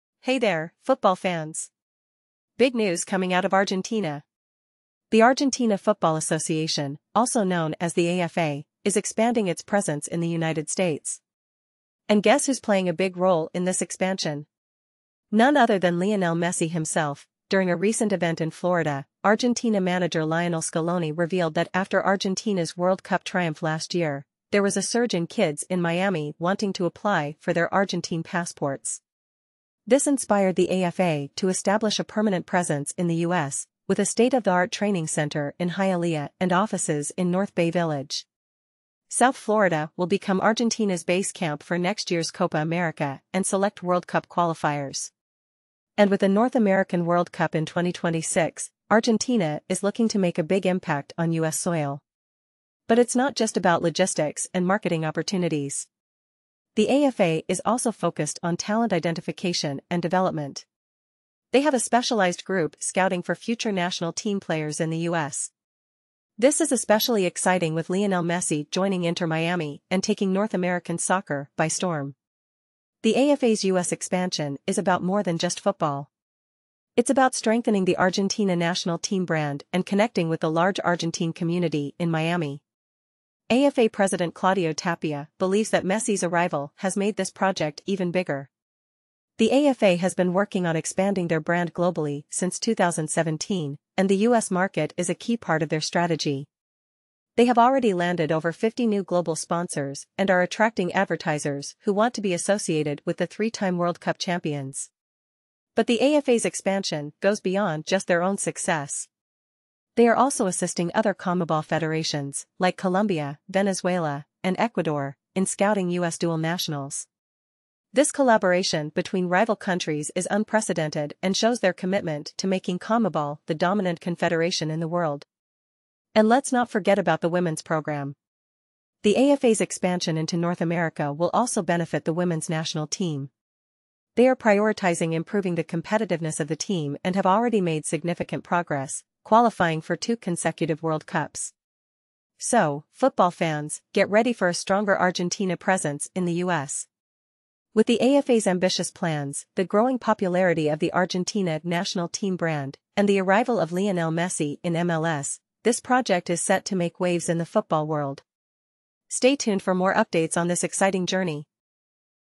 With all eyes on Messi, Argentina Football Association ramps up U.S. expansion
The Argentina Football Association (AFA) has announced its plans to expand its presence in the United States with the establishment of AFA USA. The project includes the construction of a high-performance training center in Hialeah, Florida, and commercial, administrative, and coaching education offices in North Bay Village. The AFA aims to use South Florida as its base camp for the upcoming Copa América, select World Cup qualifiers, and the 2026 World Cup. The expansion is not only focused on logistics and marketing opportunities but also on talent identification and development, with the AFA expressing excitement about scouting future national team players in the US. The AFA's move comes at an opportune time with Lionel Messi joining Inter Miami and increasing the relevance of the Argentina national team brand in the US. The AFA has also landed over 50 new global sponsors since embarking on a global campaign to expand its brand in key markets in 2017. The AFA's presence in Hialeah is expected to benefit the city's public park projects and overall economic platform. The AFA will also assist other CONMEBOL federations, such as Colombia, Venezuela, and Ecuador, in scouting US dual nationals.